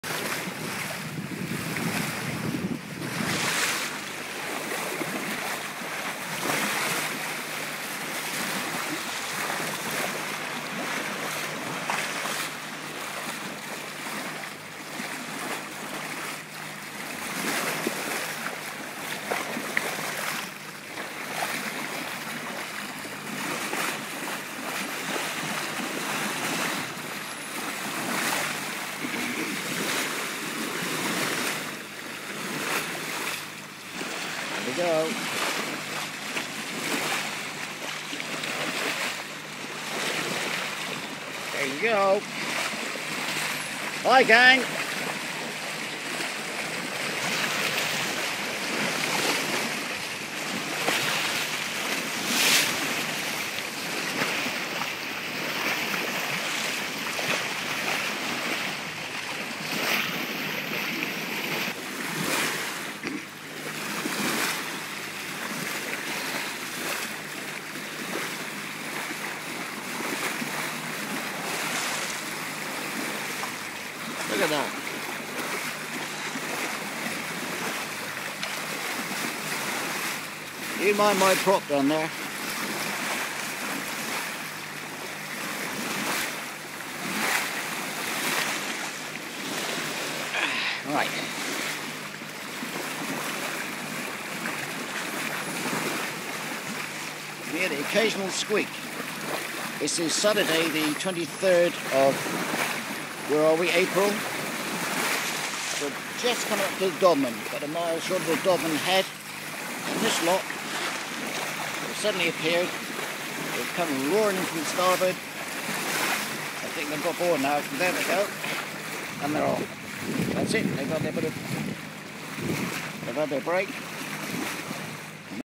Common dolphins off the Dodman. 23rd April 2011.
Filmed on my iPhone 23rd April '11.  At about 2030 I was about 2miles SW of the Dodman Point in Cornwall, UK, heading SE when I saw about 15 - 20 dolphins way off to starboard.  They came in under my bow & swam there for about 30 mins then went back out to sea again, best sighting I have ever had.